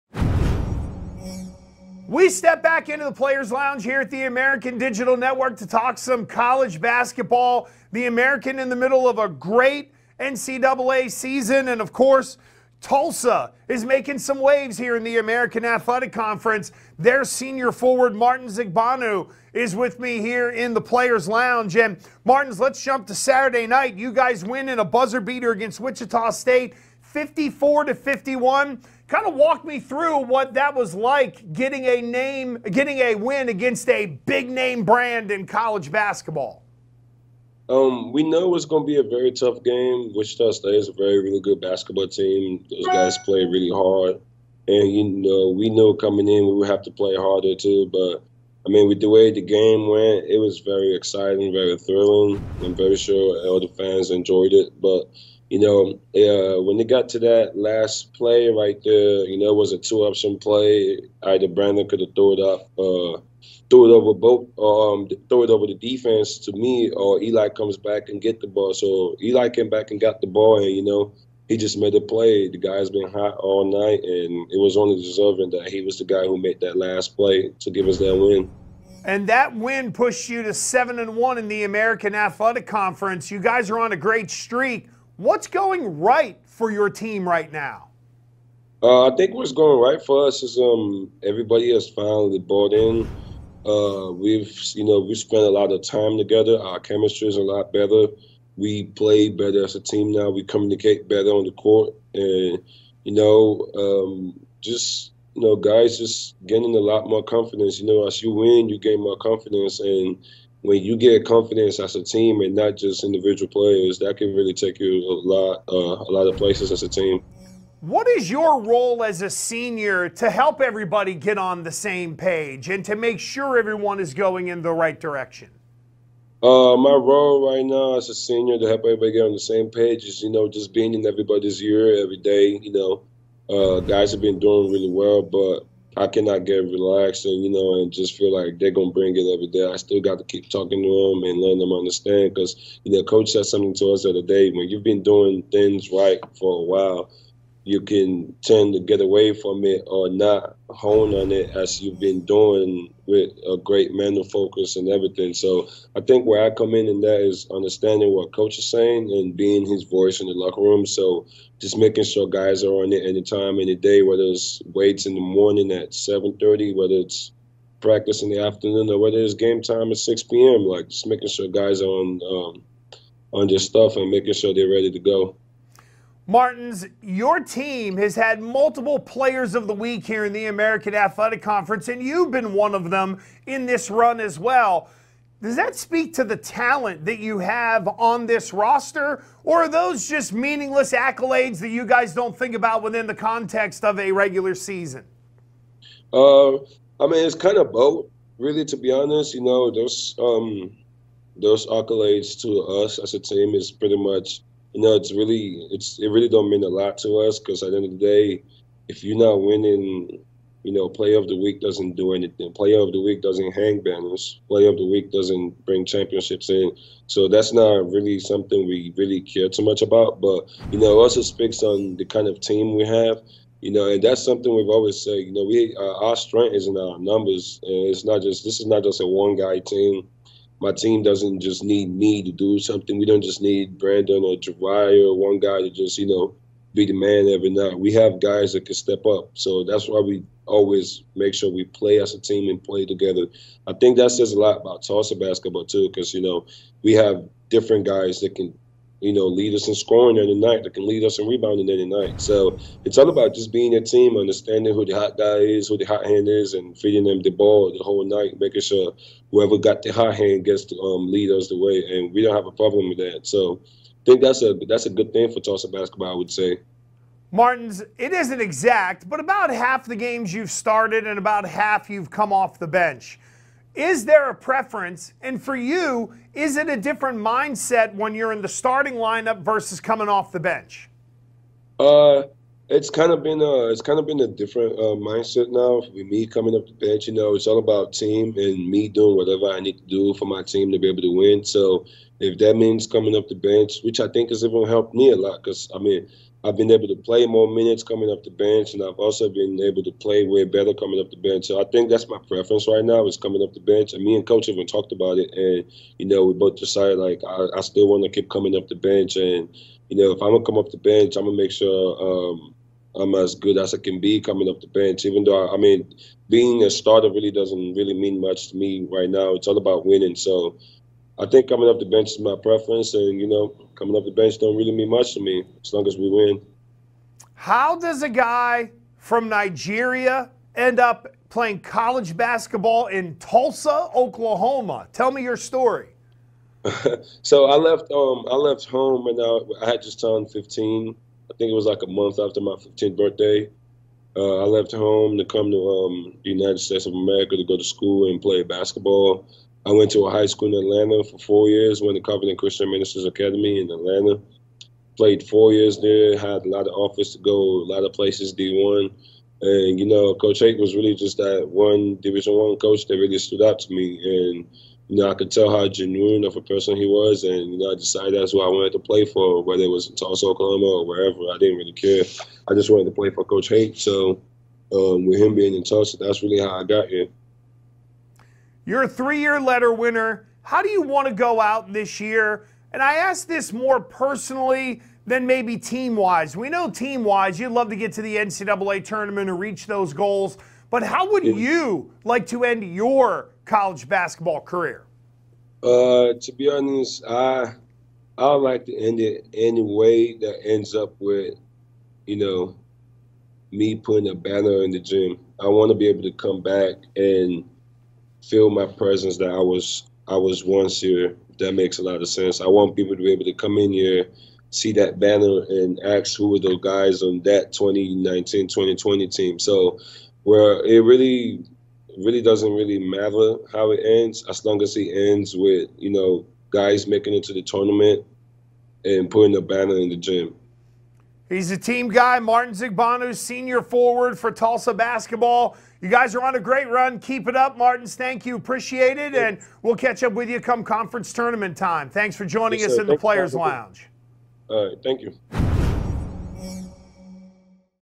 The Players' Lounge with Tulsa Forward Martins Igbanu
We step back into The Players' Lounge with Tulsa forward Martins Igbanu.